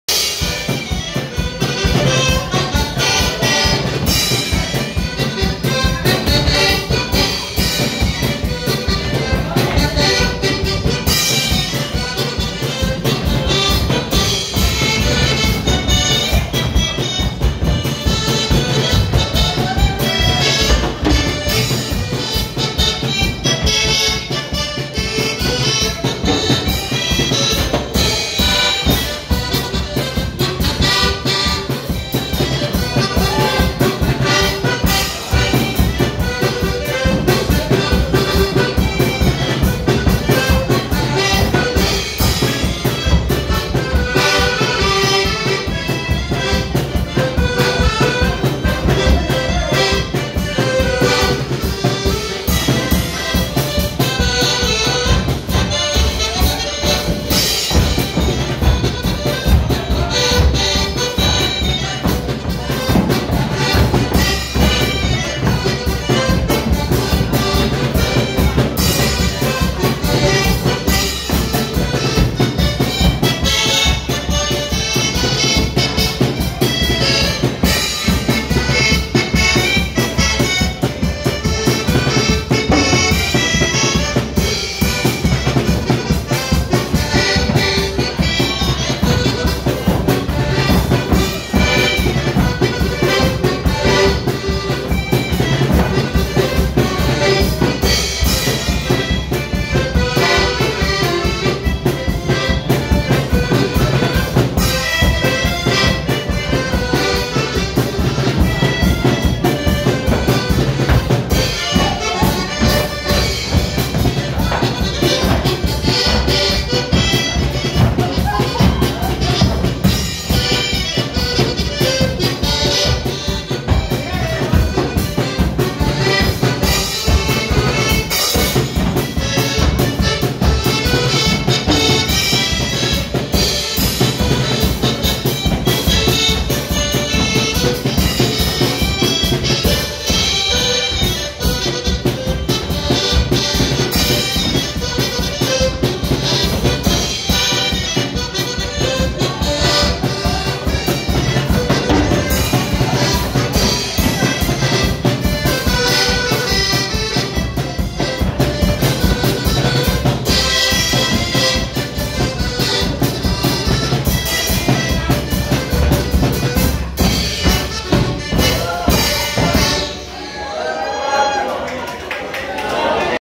Poleczka kapeli Krzysztofa Rokicińskiego rozbawiła gości w Sycynie.
Potańcówka w remizie w Sycynie, gra kapela Krzysztofa Rokicińskiego. ZAPRASZAM DO SUBSKRYPCJI KANAŁU.